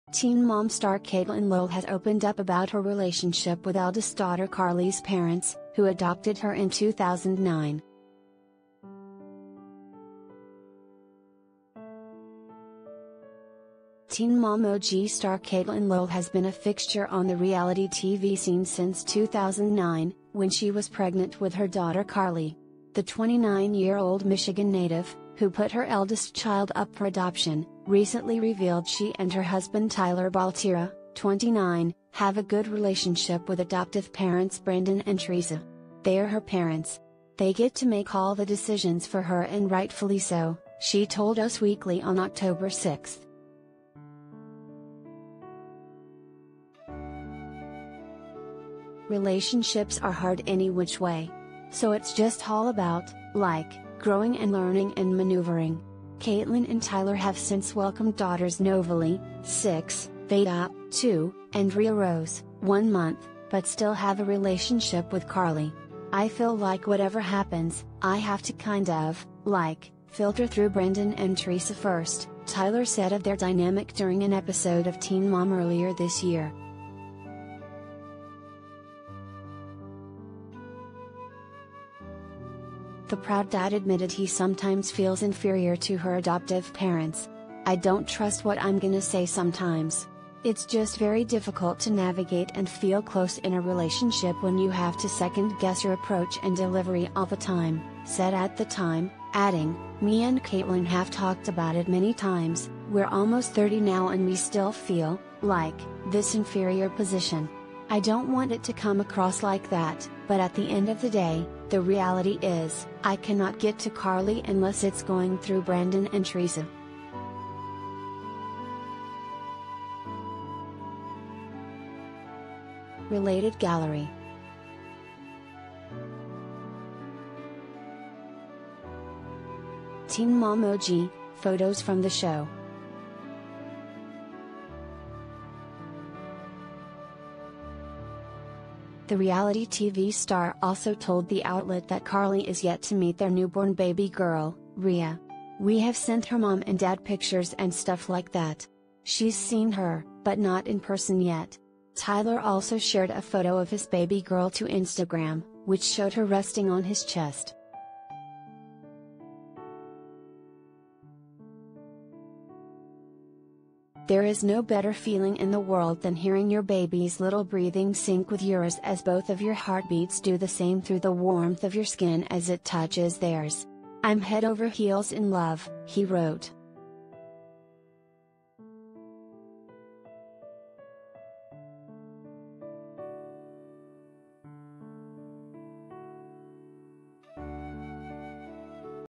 Catelynn Lowell Reveals Where She Now Stands With Daughter Carly’s Adoptive Parents
‘Teen Mom’ star Catelynn Lowell has opened up about her relationship with eldest daughter Carly’s parents, who adopted her in 2009.
Teen Mom OG star Catelynn Lowell has been a fixture on the reality TV scene since 2009, when she was pregnant with her daughter Carly. The 29-year-old Michigan native, who put her eldest child up for adoption, recently revealed she and her husband Tyler Baltierra, 29, have a “good relationship” with adoptive parents Brandon and Teresa. “They are her parents. They get to make all the decisions for her and rightfully so,” she told Us Weekly on October 6.
“Relationships are hard any which way. So it’s just all about, like, growing and learning and maneuvering.” Catelynn and Tyler have since welcomed daughters Novalee, 6, Vaeda, 2, and Rya Rose, 1 month, but still have a relationship with Carly. “I feel like whatever happens, I have to kind of, like, filter through Brandon and Teresa first,” Tyler said of their dynamic during an episode of Teen Mom earlier